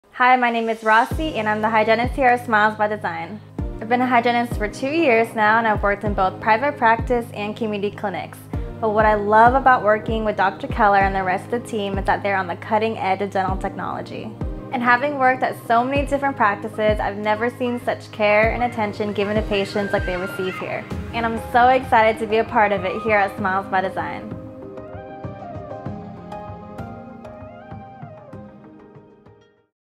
Meet Raussie — Our New Dental Hygienist at Smile by Design
Want to meet our dental team? In this video, we’ll introduce you to Raussie, our friendly dental hygienist. With over 2 years of experience, Raussie is ready to help you maintain the best oral health possible.

SMILES BY DESIGN - REJUVENATING IMPLANT DENTISTRY
830 6th St. South